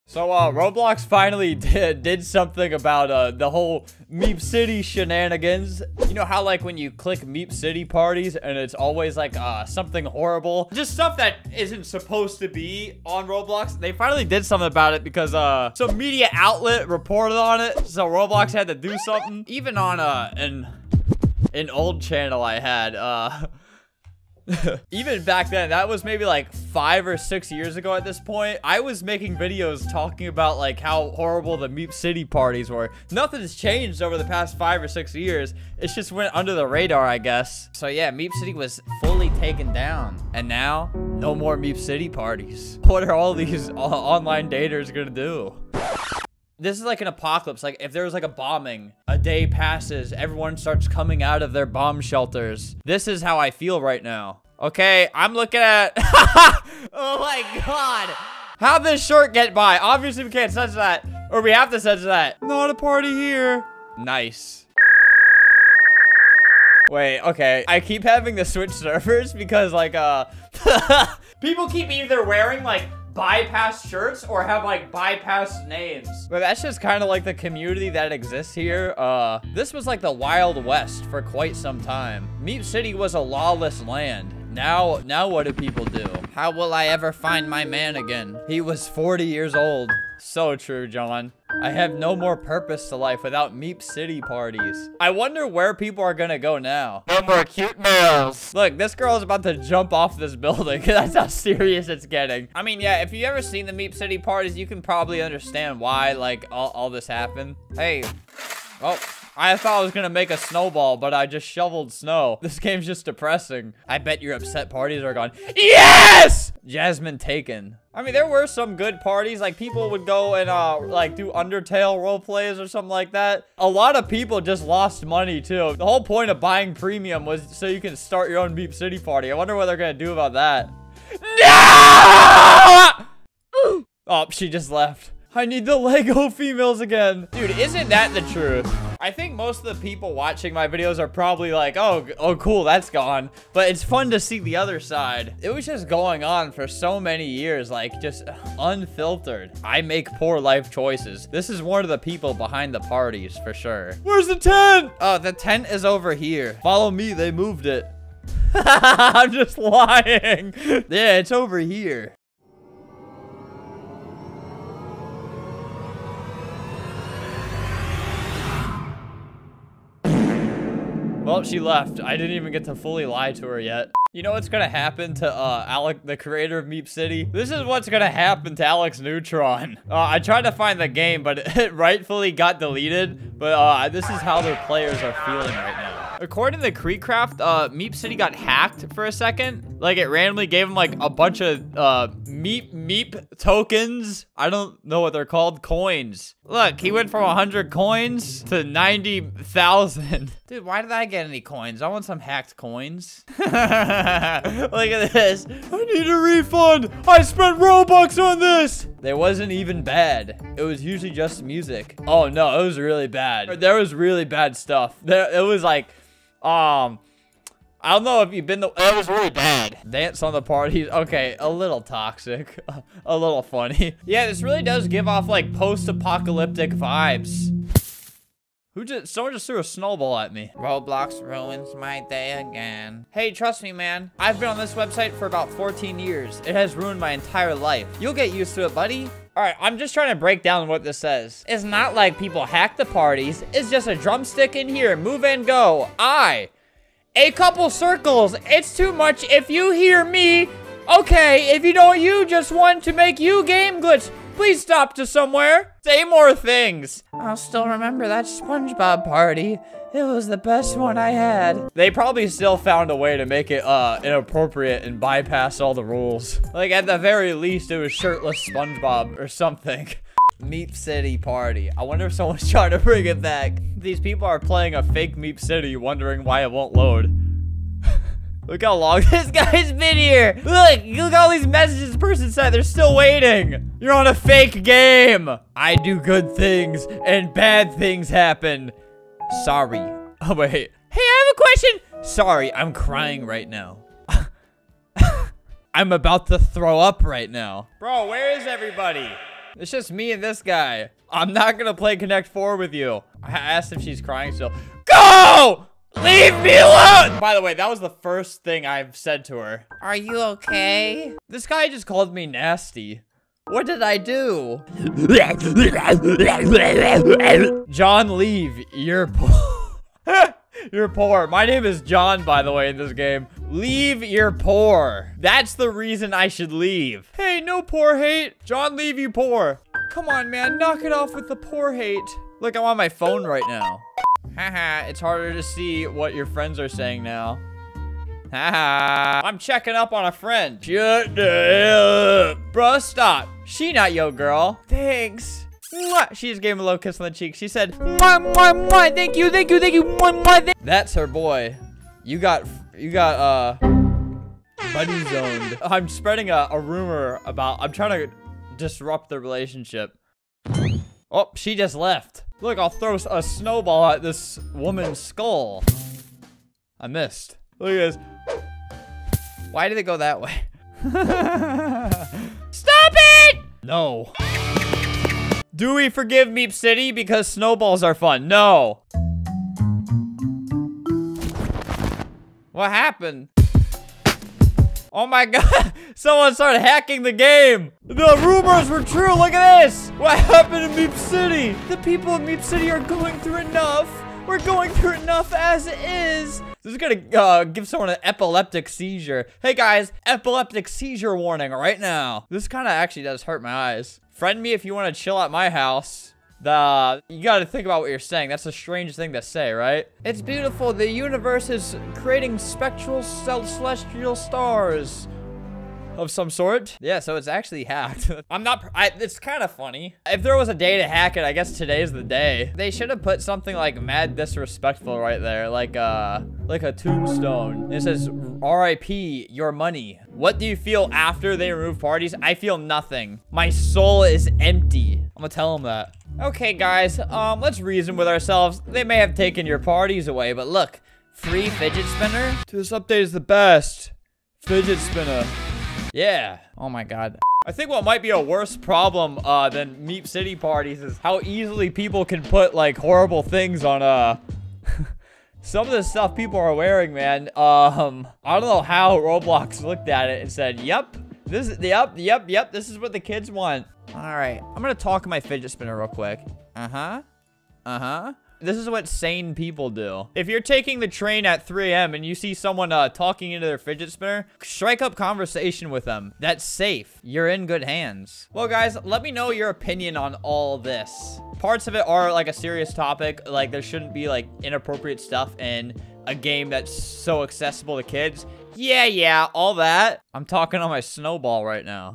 Roblox MEEP CITY was SHUTDOWN LOL
today i talk about how meep city got shut down for a bit over a problem that's been going on for years now... what r ur thoughts